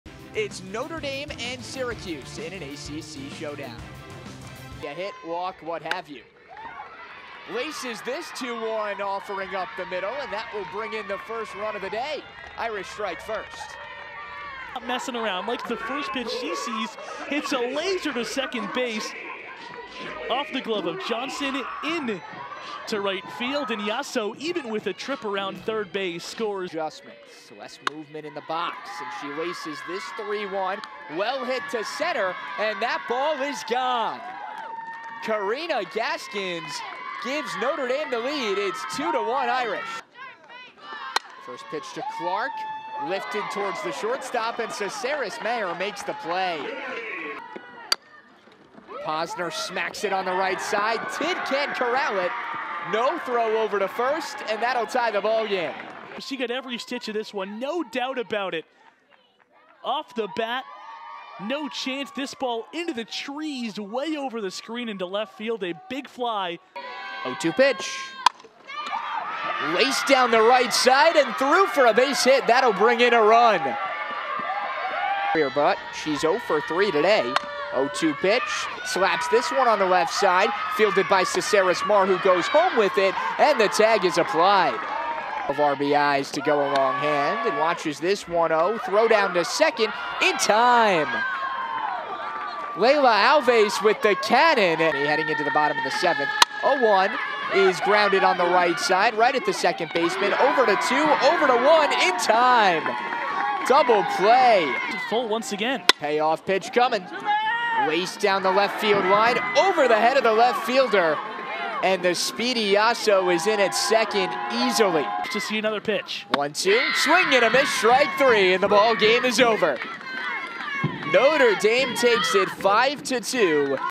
Highlights | Syracuse vs. Notre Dame
Highlights from Syracuse softball's 5-2 loss vs. Notre Dame in game one of the three-game series.